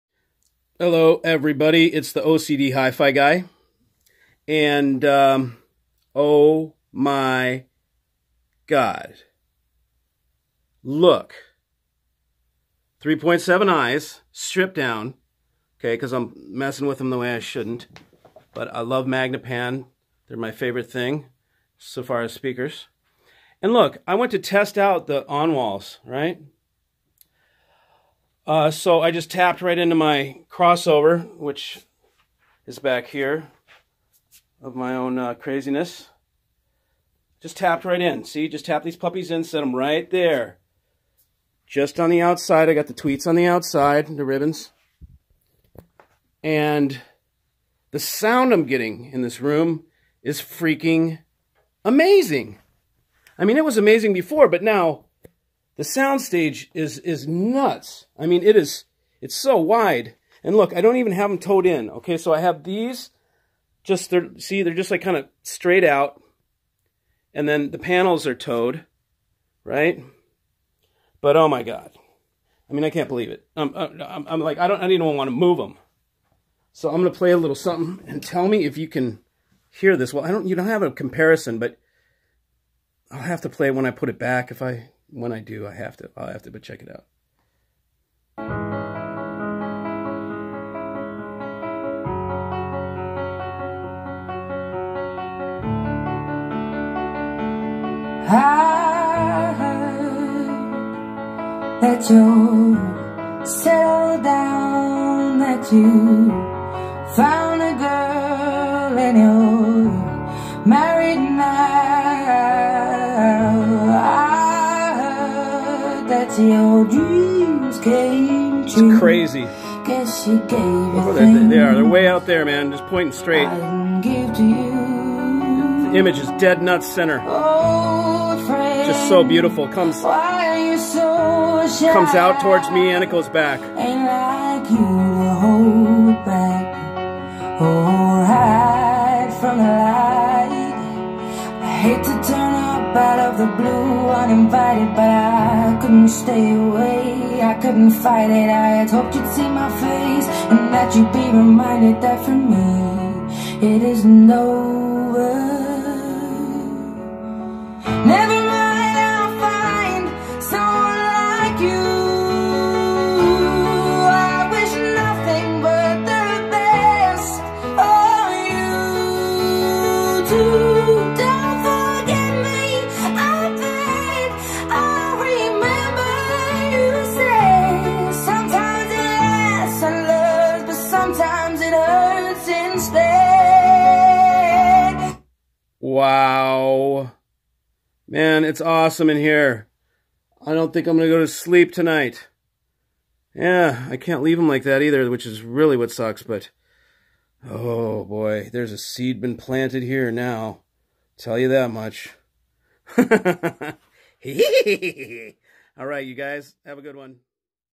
Magnepan hallucinatory imaging accidentally
Testing my onwalls leaned against 3.7i just to check operation and lo and behold someone slipped me a Mickey... or maybe a Timothy..